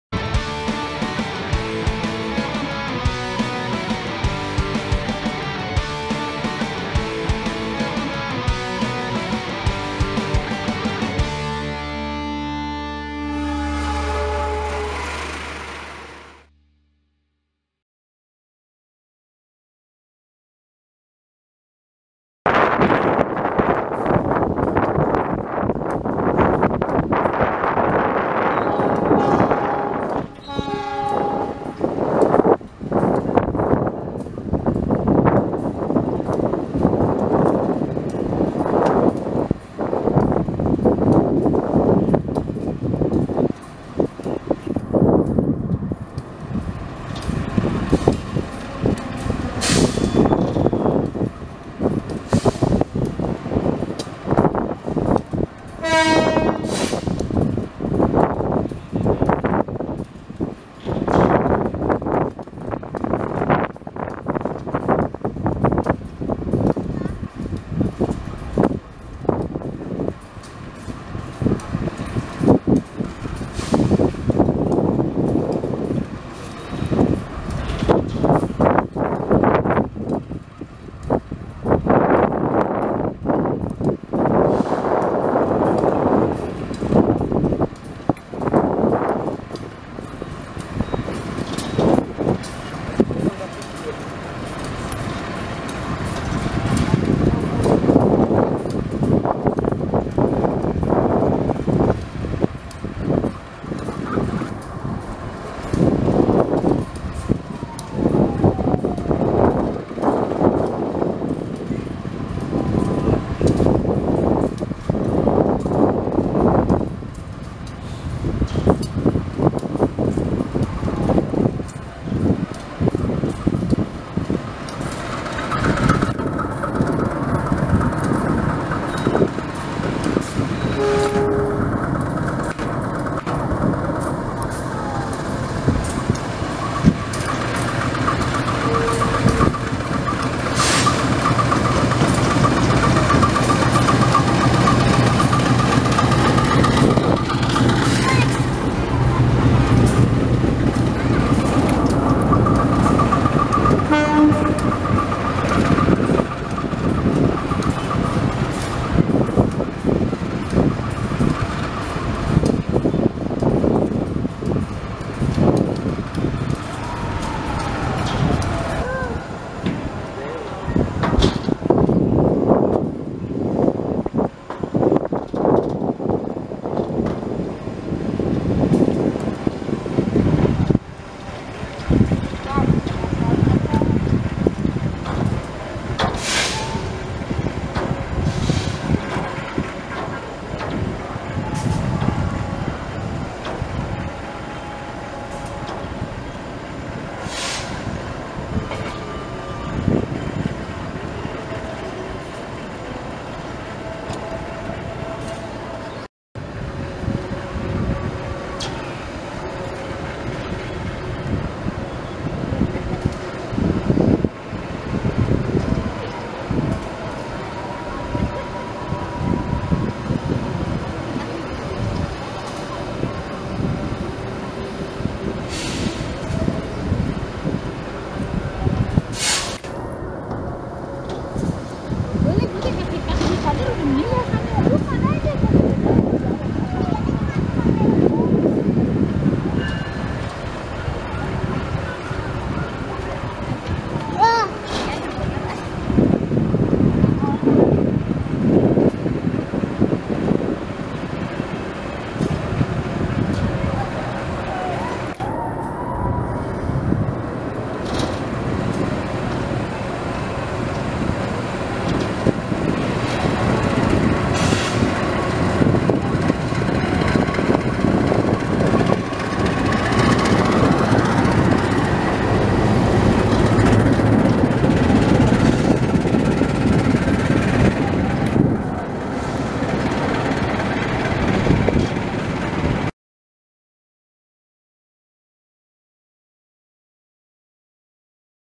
Inilah Proses Bagaimana Lokomotif Diputar Arahnya menggunakan Meja Pemutar
Cara memutar posisi kabin lokomotif menggunakan turn table